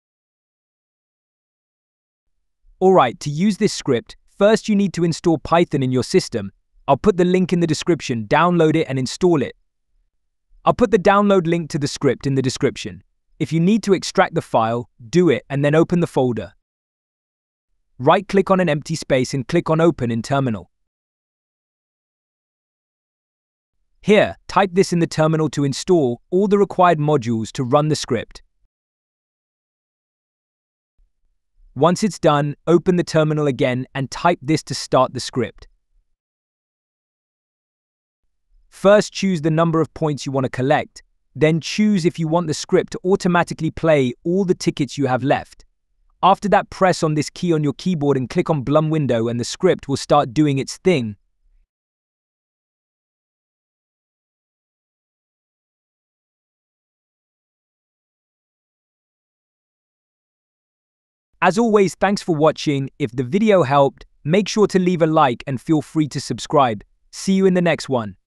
Blum Auto Clicker Script - Auto Blum Mini Game Clicker Bot (200+ POINTS PER GAME!!) PC METHOD
Using any sort of cheat puts your account at risk!
This puts your account at the risk of getting banned!